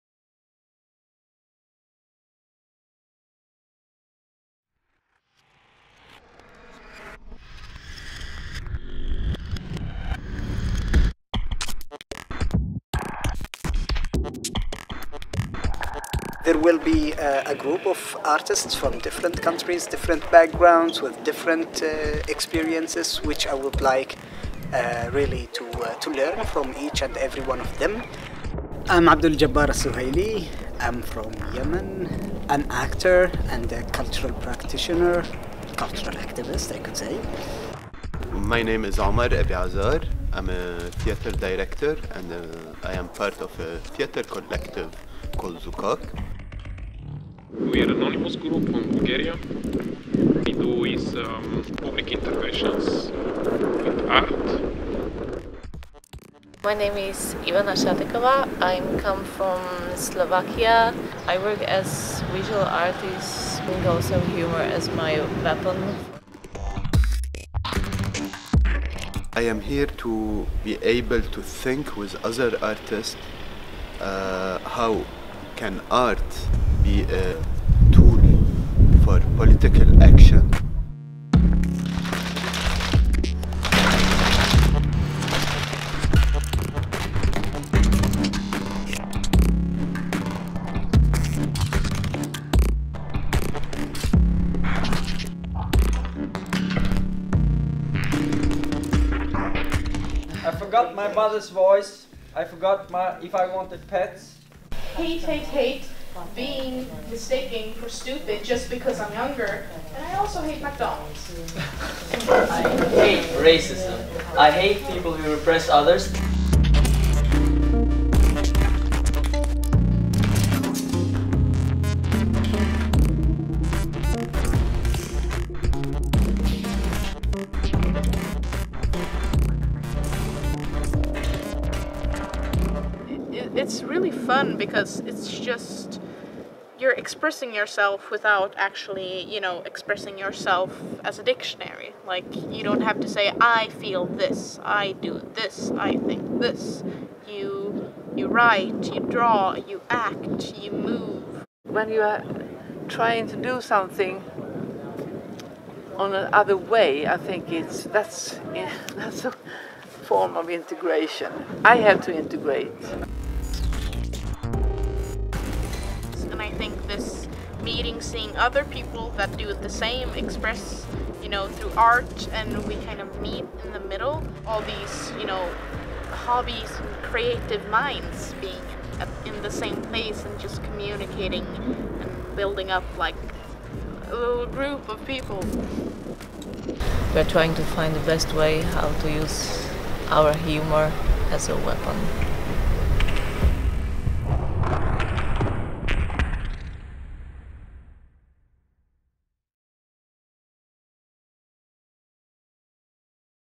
Borderline Offensive [Artistic Residency, Sweden]
4 artists worked together between 1-10 June 2018 to reflect about how can humor and art play a role in addressing cultural conflicts in contemporary society and intermediating peaceful dialogue between the different communities living in Europe (for a long time or newly-arrived). This included a 2-day workshop for a mixed group of around 20 young locals and migrants . Artists were Abduljabbar Alsuhili (SE), Destructive Creation (BG), Ivana Satekova (SK), Omar Abi Azar (LB).
FIlm/editing by Anna Hulth